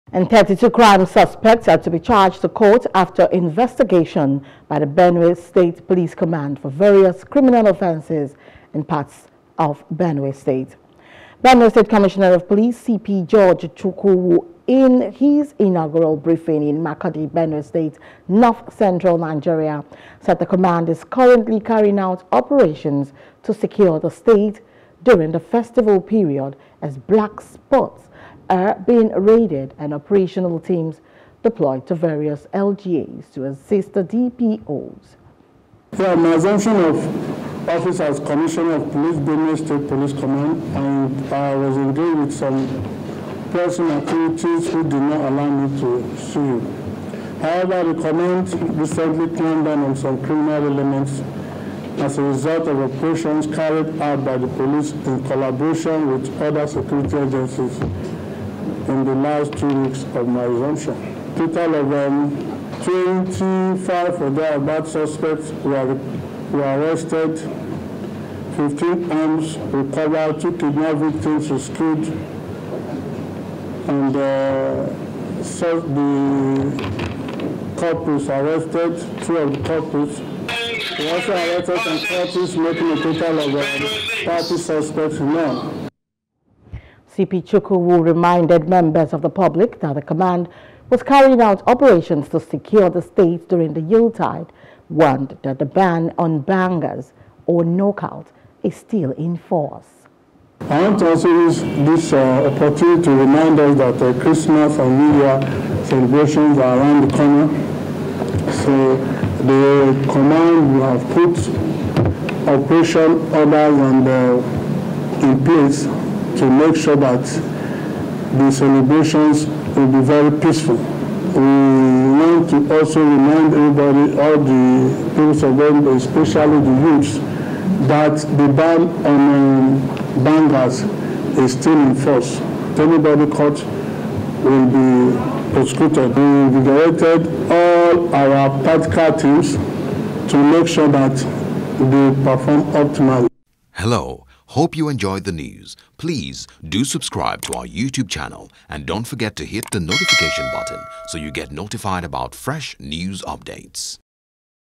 Benue: State Police Command To Prosecute Over 40 Crime Suspects
32 crime suspects are to be charged to court after investigation by the Benue state police command for various criminal offences in parts of the Benue State.